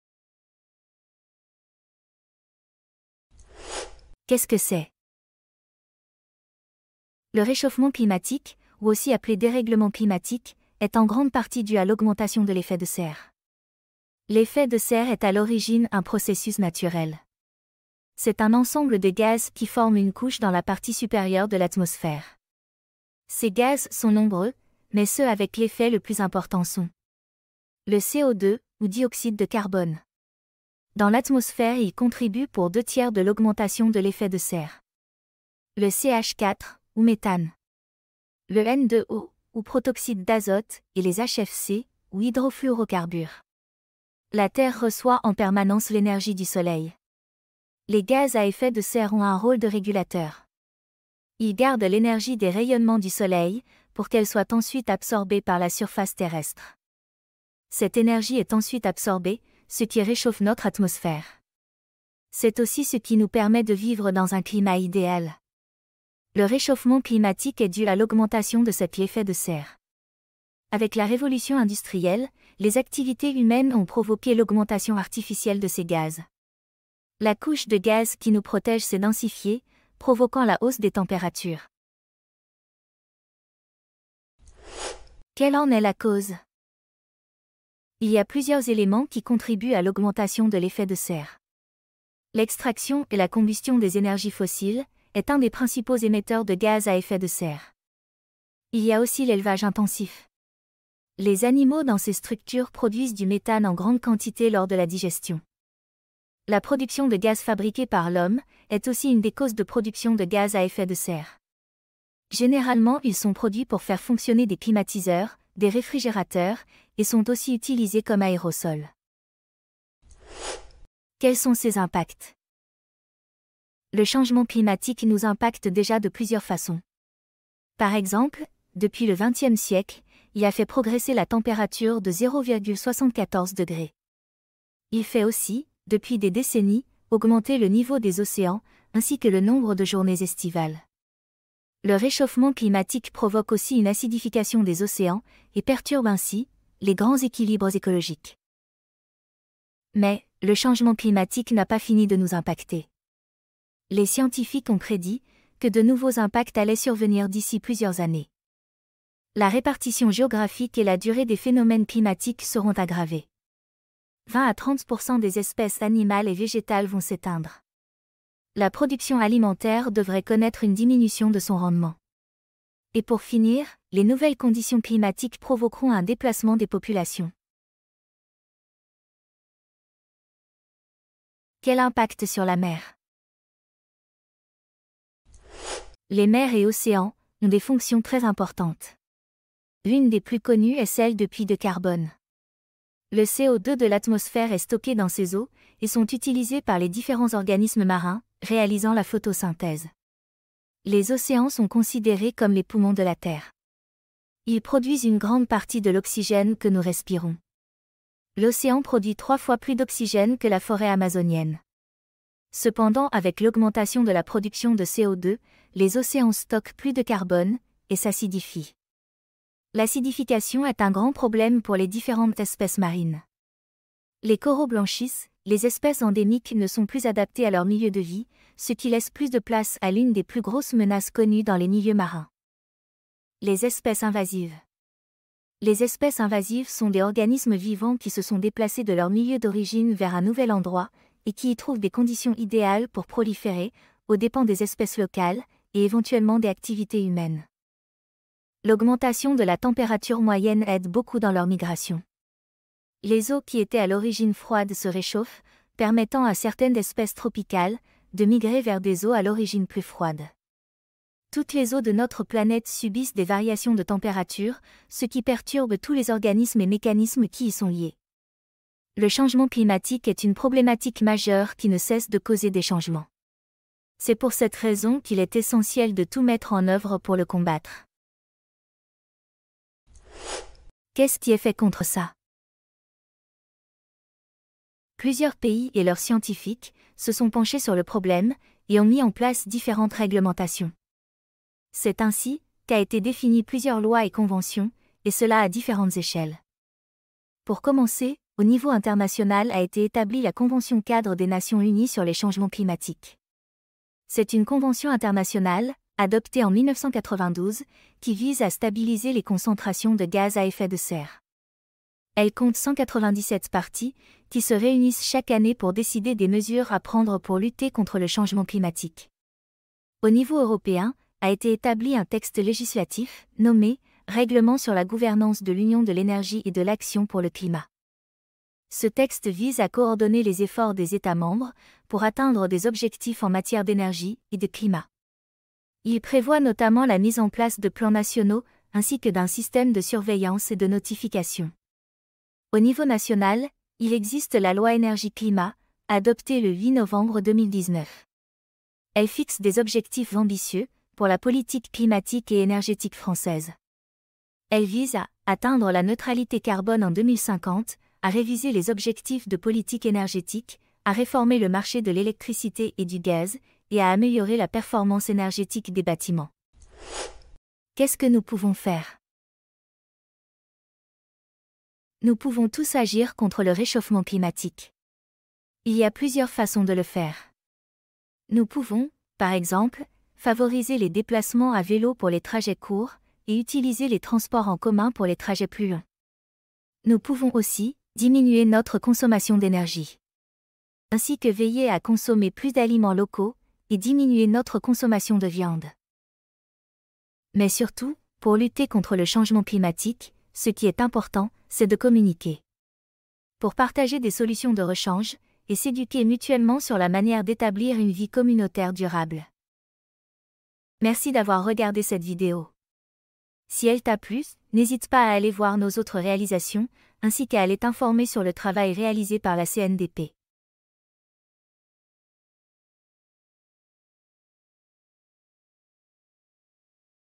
La mer en débat en Méditerranée : Le changement climatique, qu'est-ce que c'est ? 20/11/23-26/04/24
Vidéo réalisée dans le cadre du débat public « La mer en débat », organisé par la Commission nationale du débat public (CNDP) du 20 novembre 2023 au 26 avril 2024.